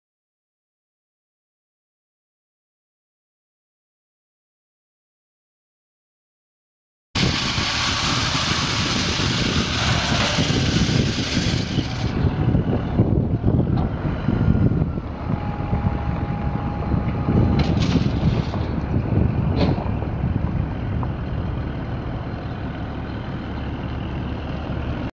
Kuhn SR110 hay rake for sale at auction | bidding closes June 6, 2018
Kuhn SR110 hay rake
20'W
Hydraulic lift
Pin hitch
Jack stand
205/75R15 tires
Still in use
Serial E2807
It's located in Red Oak, OK, and we're selling it to the highest bidder regardless of price.